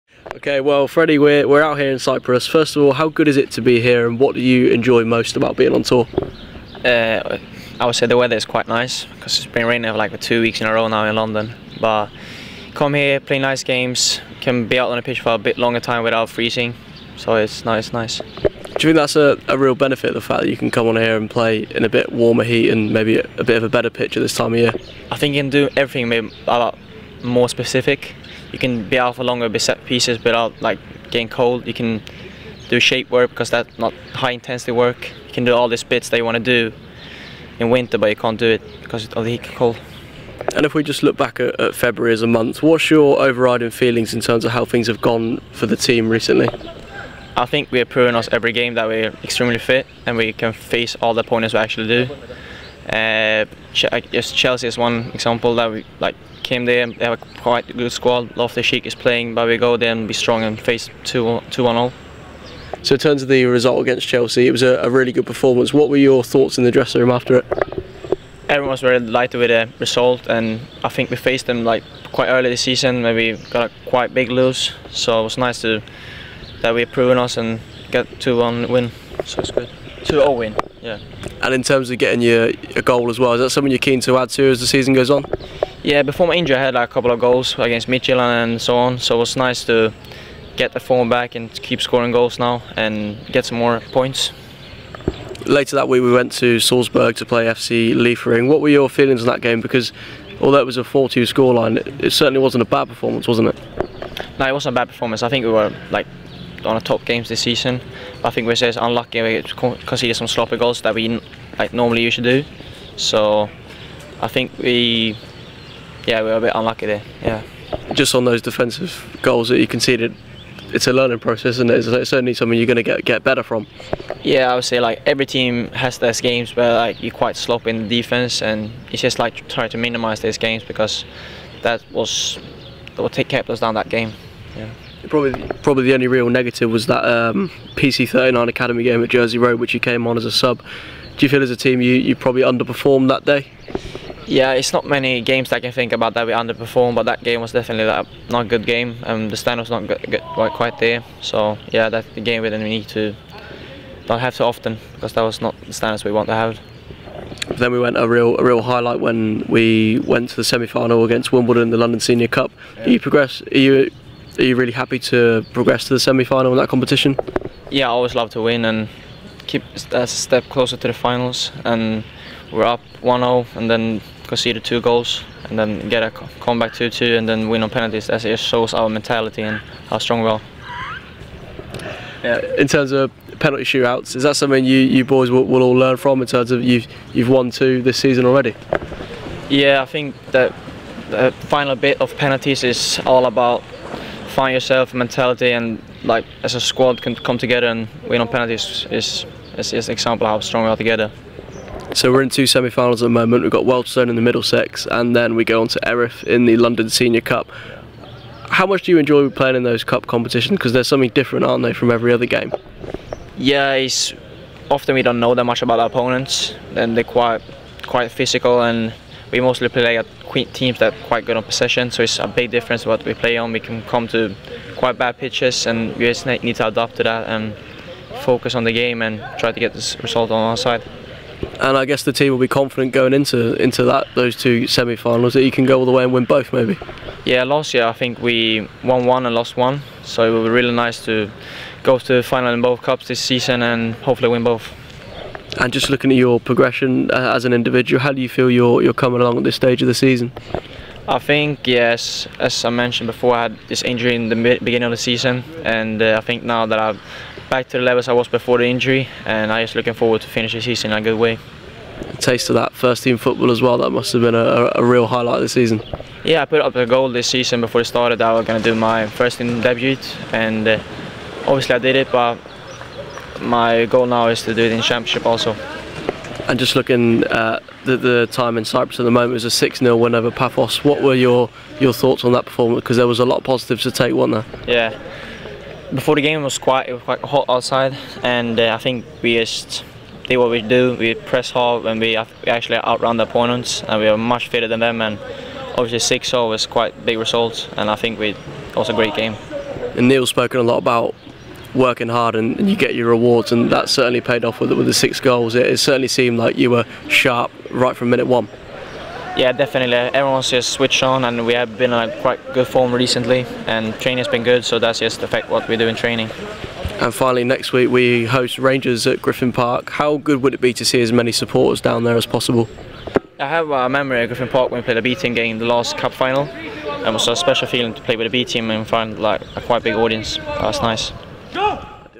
Brentford B: B Team Monthly with Fredrik Hammar
The young Swede discusses February and how things are going with Brentford B.